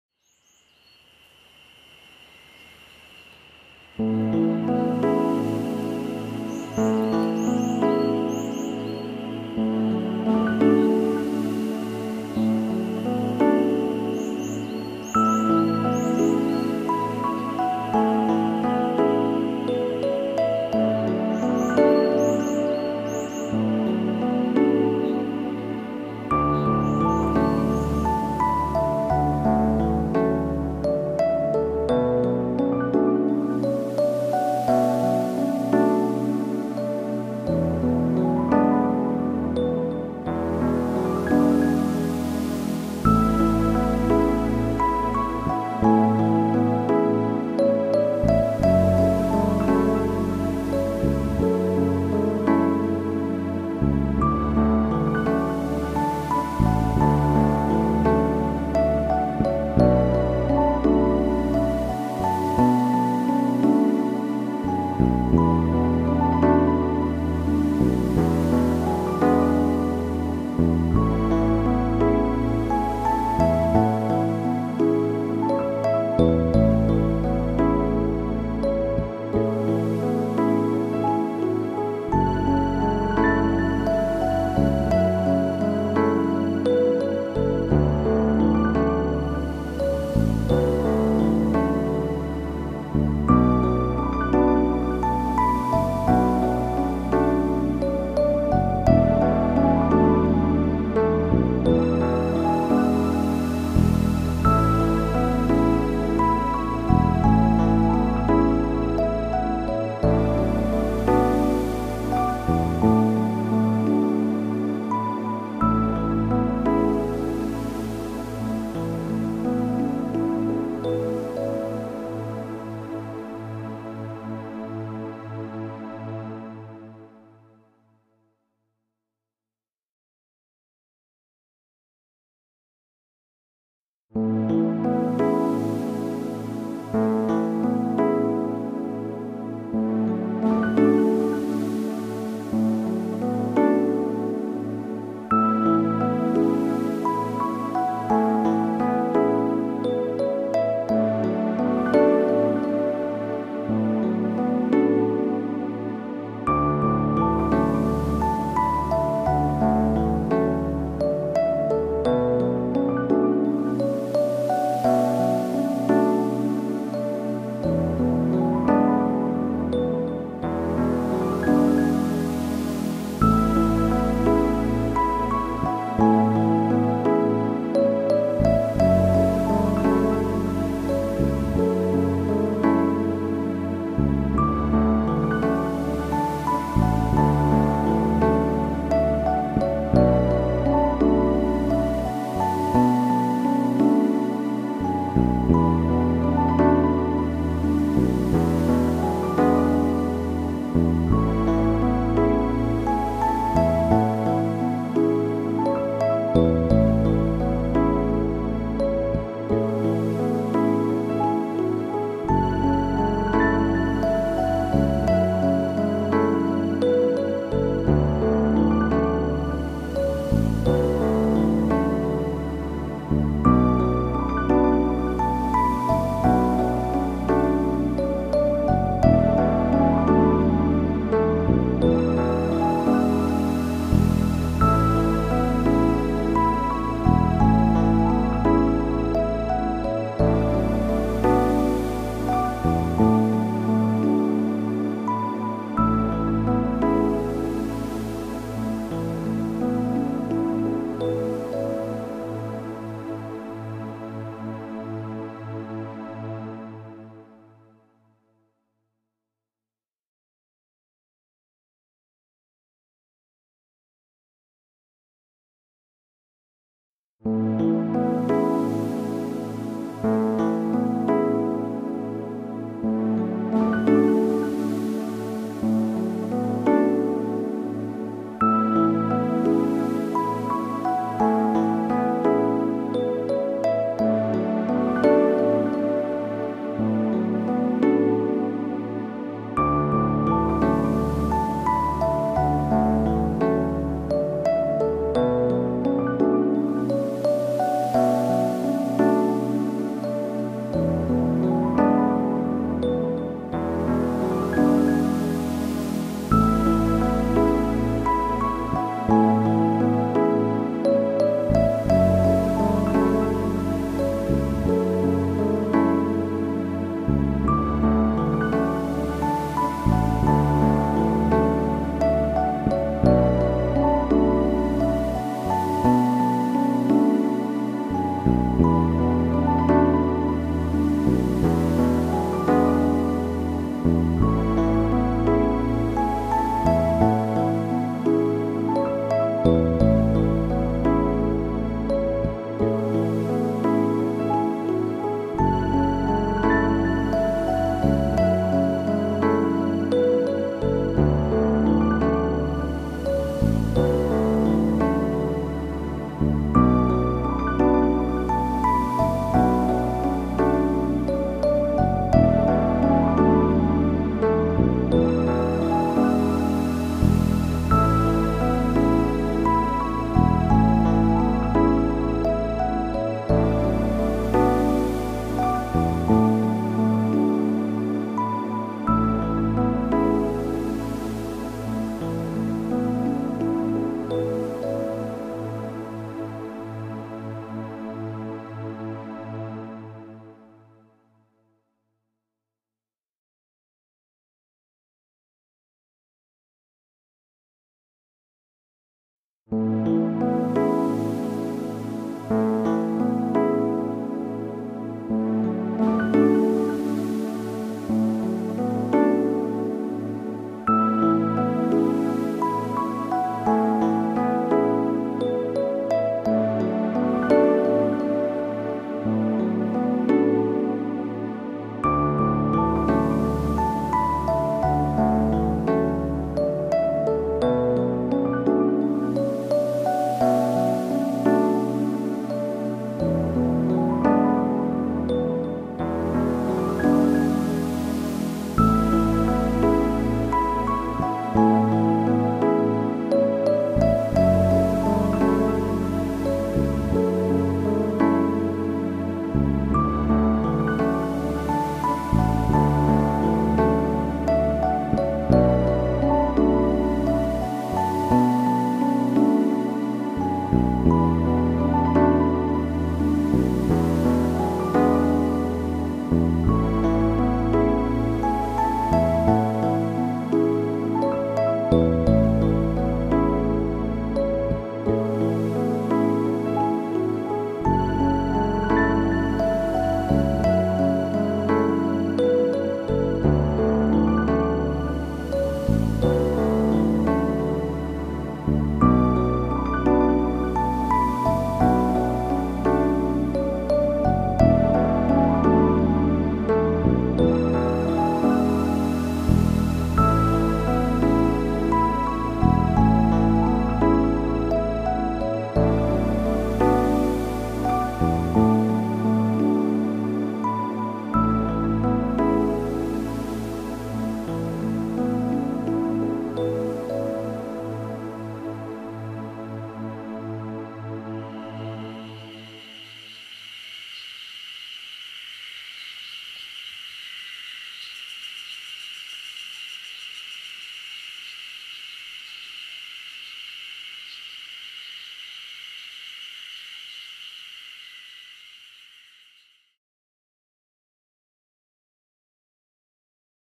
美しき新緑の志賀高原・4K
志賀高原に新緑が美しい季節がやって来ました。今回は澗満滝から一沼、丸池、渋峠、奥志賀と、それぞれの場所の風景を美しい新緑とコラボさせました。撮影日2021年6月5日